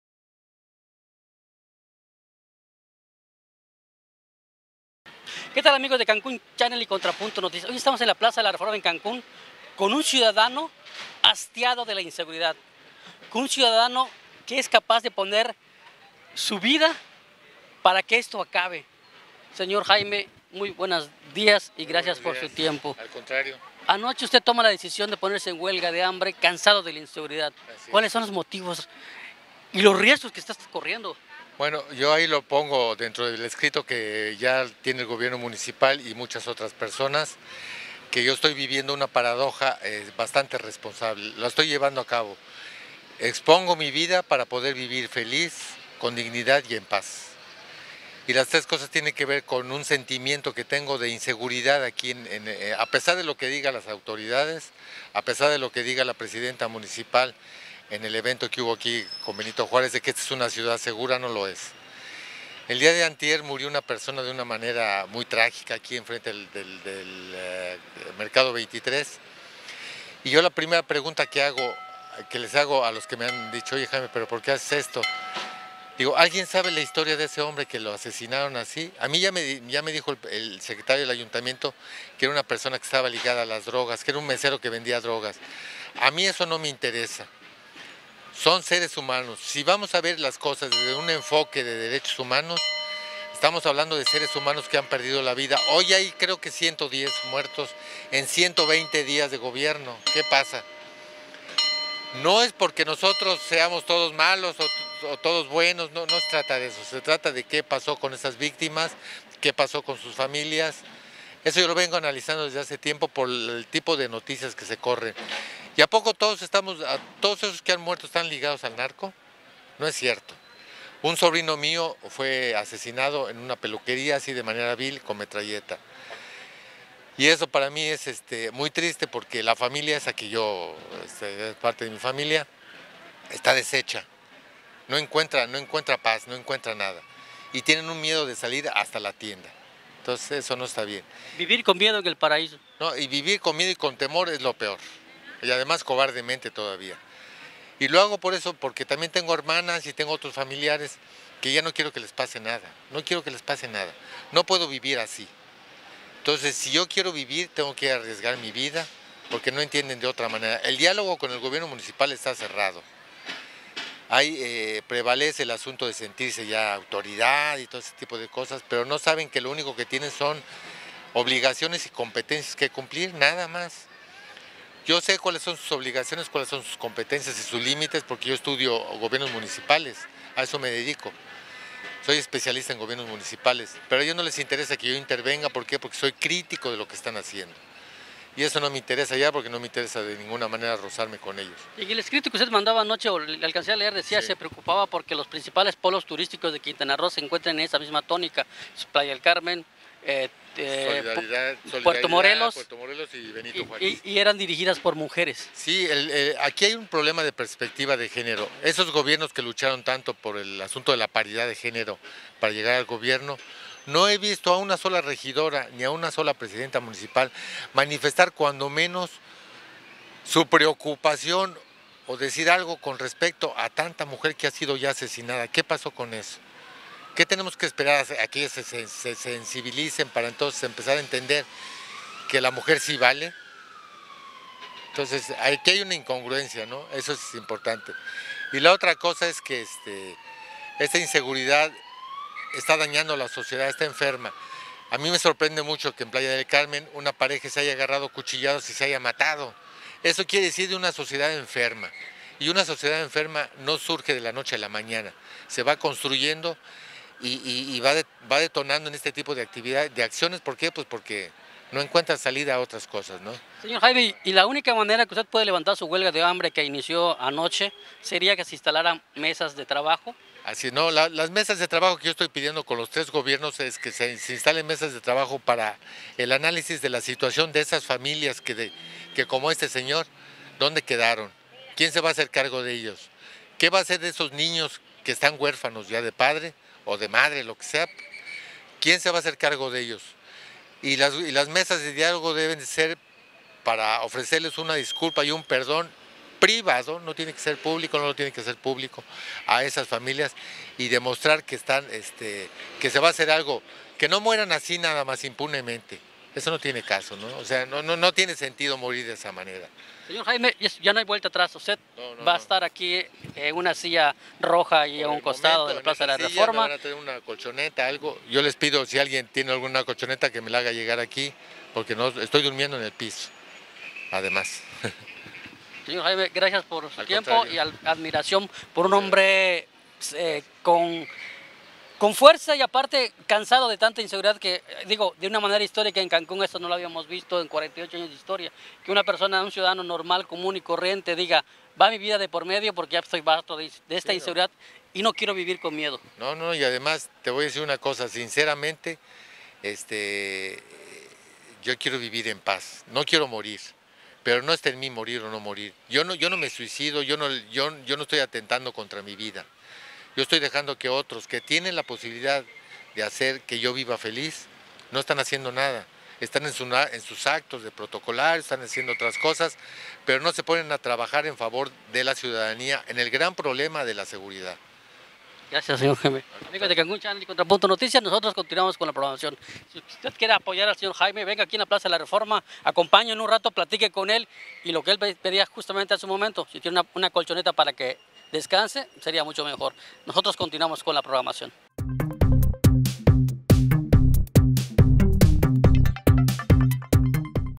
CANSADO DE VIVIR CON MIEDO, CANCUNENSE SE PONE EN HUELGA DE HAMBRE POR INSEGURIDAD
“Pongo en peligro mi vida, porque quiero vivir con dignidad, en paz y feliz”, dice Jaime Enrique Rendón de León, y afirma que “por supuesto que no deseo morir, pero también me niego a vivir con temor y con Miedo”.